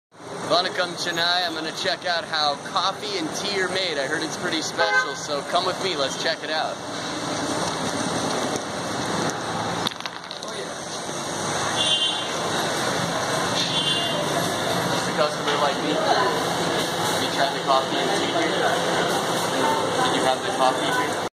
Adam Boborrow visits Chennai!
Adam Boborrow visits Chennai and checks out a tea and coffee shop!

CEAT Ultimate Table Tennis Season 1